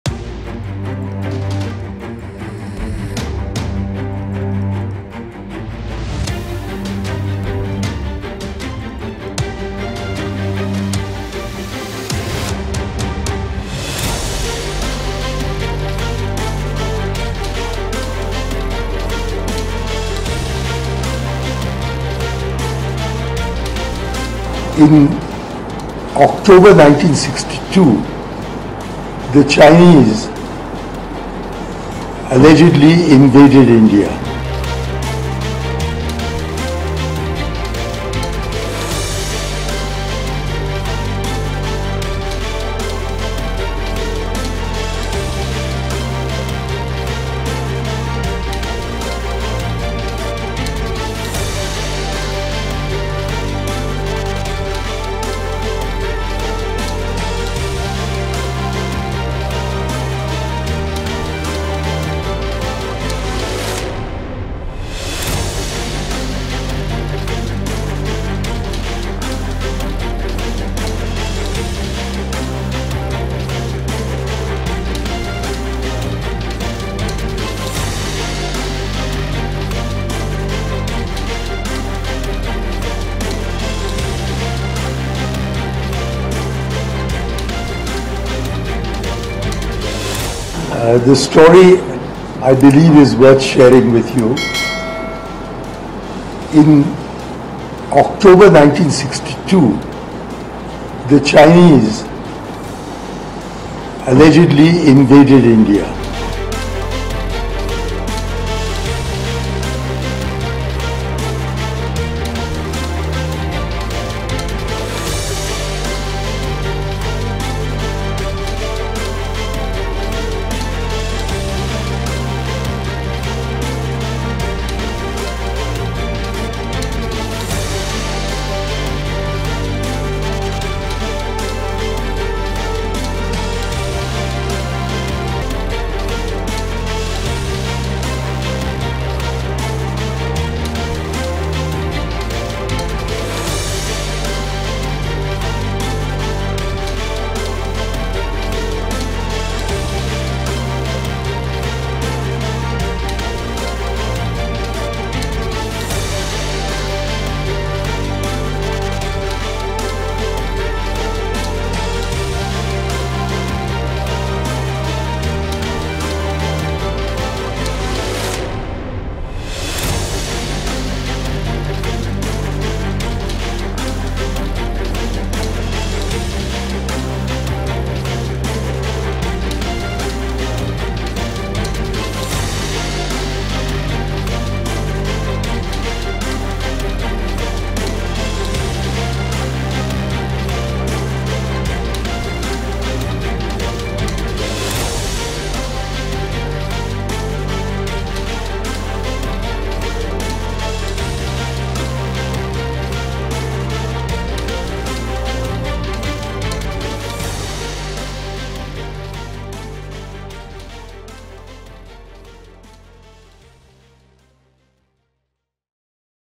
Chinese "allegedly invaded" India in 1962: Congress’ Mani Shankar Aiyar triggers fresh blunder
Congress leader and former Union Minister triggered a fresh controversy during a public event in Delhi. INC leader Mani Shankar Aiyar referred to the 1962 Indo-China war as an “alleged Chinese invasion”. Addressing the press conference on May 28, Aiyar said, "In October 1962, the Chinese allegedly invaded India”. Controversy's favourite child, Mani Shankar Aiyar has made several comments that put Congress in a spot. Aiyar had stated that ‘India must enter a dialogue with Pakistan and respect them as they possess an ‘atom bomb’